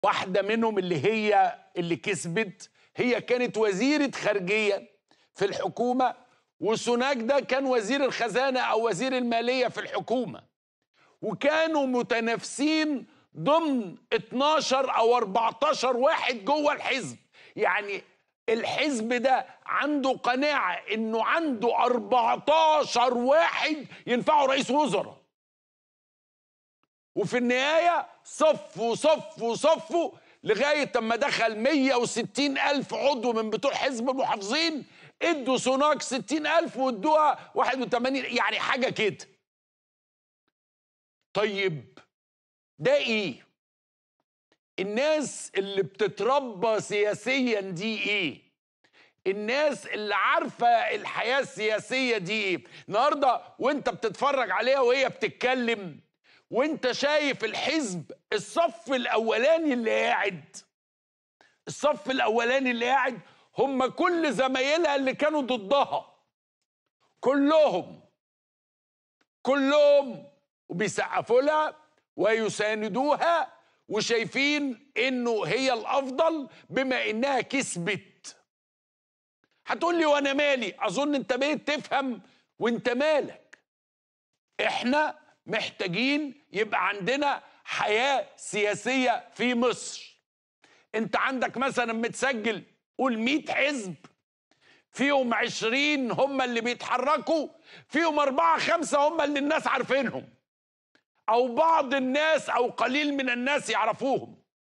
عمرو اديب: اللي كسب رئاسة وزراء بريطانيا كانت بتتنافس مع منافسين كتير جوه الحزب
عمرو اديب: اللي كسب رئاسة وزراء بريطانيا كانت بتتنافس مع منافسين كتير جوه الحزب.. فيه حياة سياسية بتفرز كوادر